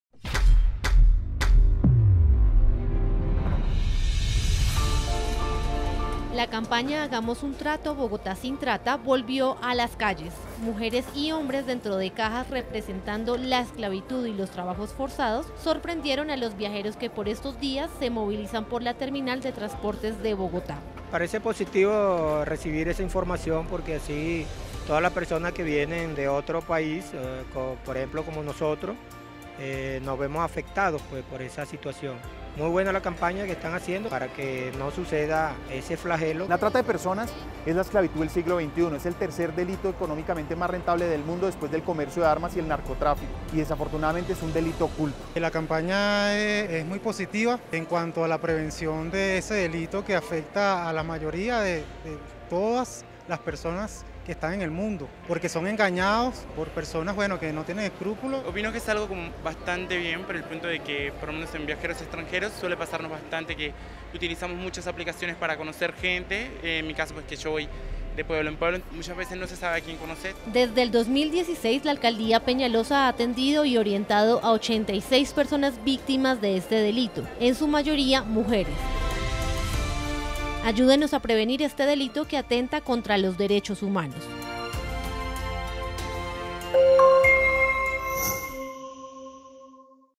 Bogotá sin trata en la Terminal de Transportes
En Semana Santa, la campaña ‘Hagamos un trato, Bogotá sin trata’ se desarrolla en la Terminal de Transportes de Bogotá. Con esta estrategia, la Alcaldía de Enrique Peñalosa busca concientizar a miles de viajeros que entran y salen de la capital del país sobre el delito de la trata de personas.